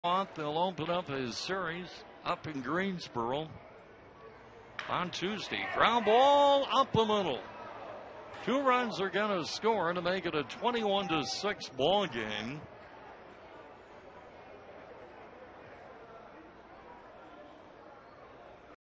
Lexington's Arteaga drives in two
4/27/14: Lexington's Humberto Arteaga singles to bring home two in a 21-6 loss to Greenville